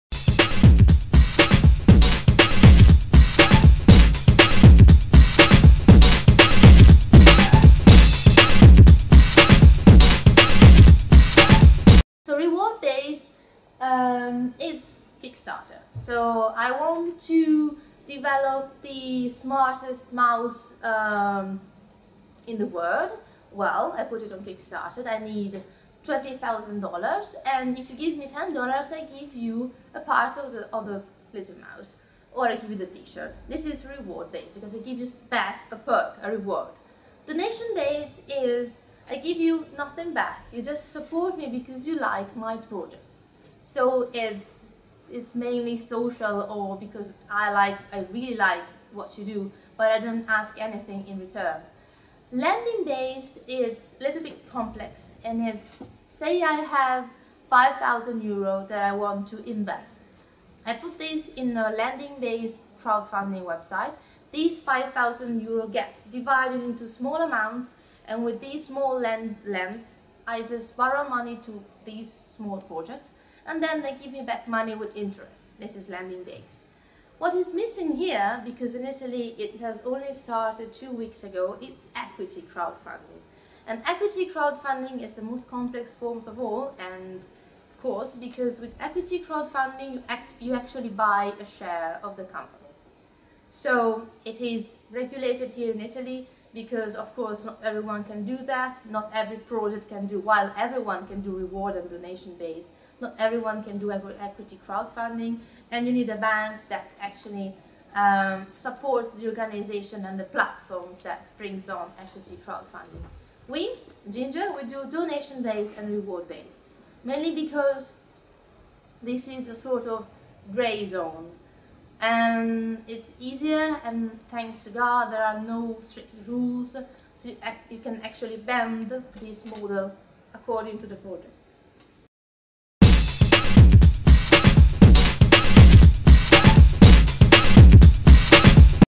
Virginia Carolfi, The four tipes of crowdfunding
In questo video Virginia Carolfi, community manager e blog-social media curator di GINGER, ci spiega i quattro tipi di crowdfounding:
REWARD BASED: si investe in un progetto in cambio di una ricompensa;
DONATION BASED: le persone investono in un progetto esclusivamente perchè  ci credono, senza avere o chiedere nulla in cambio.
LENDING BASED: persone con dei fondi economici a disposizione decidono di investirli su un sito di lending based crowfunding dove vengono divisi in cifre più piccole che finanziano poi diversi progetti. Gli stessi soldi verranno poi restituiti con interessi annessi.
EQUITY CROWDFUNDING: una persona compra una quota di un''azienda. è la tipologia più complessa ed è gestita tramite le banche.